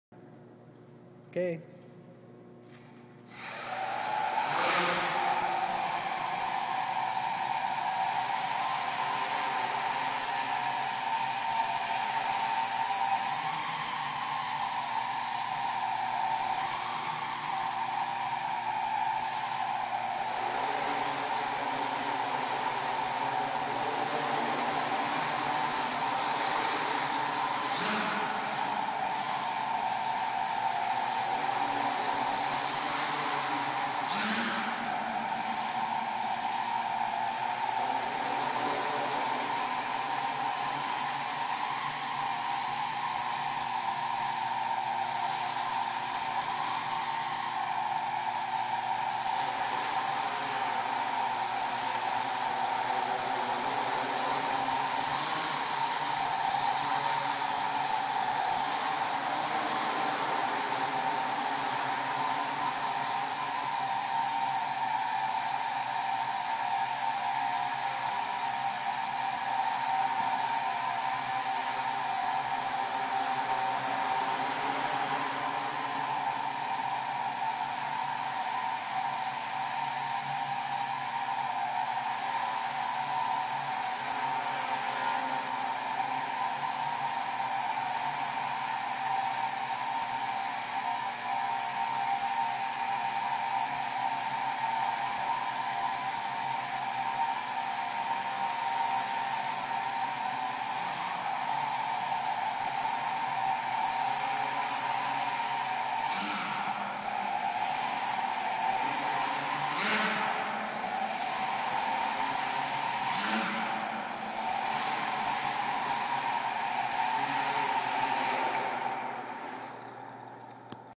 Quadcopter
V2 HK quadcopter board, e400 size 910kv motors, 10x3.8 apc props, 2650mah value hobby lipo, funfly hobbywing 18a ESCs, 480watts peak  total.